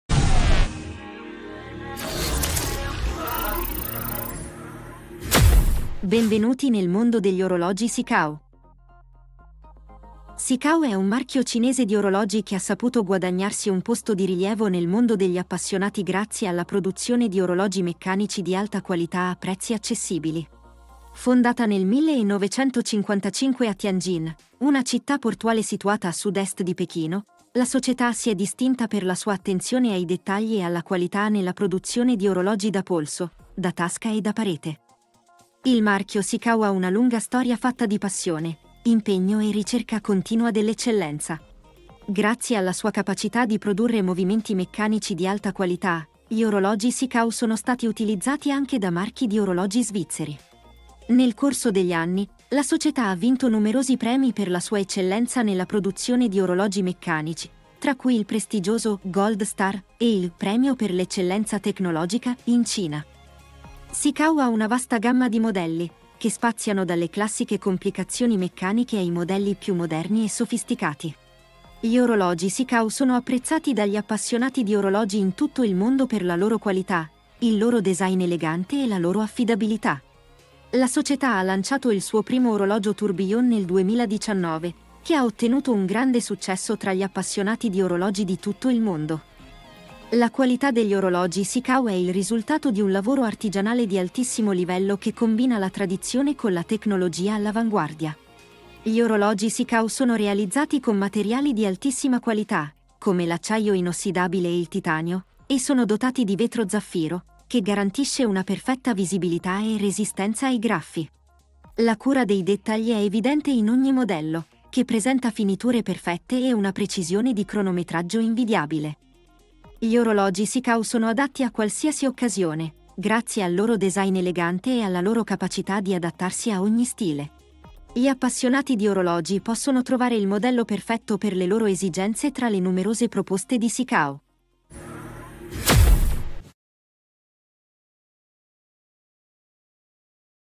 Eleganza Meccanica: Scopri l'Arte degli Orologi Sea-Gull
Tra le innumerevoli maison orologiere, Sea-Gull brilla come una stella nel firmamento dell'orologeria meccanica. Fondata nel 1955 a Tianjin, in Cina, questa prestigiosa casa orologiera è famosa per la sua abilità nel combinare l'arte tradizionale dell'orologeria con l'innovazione tecnologica, creando orologi di alta qualità dal design affascinante.
Gli orologi Sea-Gull sono il frutto di un'impareggiabile maestria orologiera e una passione per la perfezione. Ogni orologio è assemblato con meticolosità da orologiai altamente qualificati, che dedicano ore di lavoro a creare movimenti meccanici impeccabili.
La collezione di orologi Sea-Gull offre una vasta gamma di modelli, dalla sobria eleganza degli orologi da polso classici agli intricati tourbillon. Questi orologi non solo offrono prestazioni accurate e affidabili, ma sono anche veri pezzi d'arte che catturano l'attenzione con il loro design unico e raffinato.
La maison cinese si è guadagnata una solida reputazione nel mondo dell'orologeria, grazie alla sua dedizione per l'innovazione. Sea-Gull ha dimostrato di essere all'avanguardia nella tecnologia e nella produzione di orologi di alta qualità, con una visione di futuro che continua a stupire gli appassionati di orologi di tutto il mondo.
Il video su YouTube dedicato agli orologi Sea-Gull ti offre l'opportunità di scoprire da vicino l'arte e la precisione che si celano dietro ogni orologio.
Se sei un amante dell'orologeria meccanica e cerchi un orologio che unisca eleganza, innovazione e qualità, gli orologi Sea-Gull sono ciò che fa per te. Scopri l'affascinante mondo degli orologi Sea-Gull e lasciati conquistare dalla maestria e dalla bellezza degli orologi meccanici.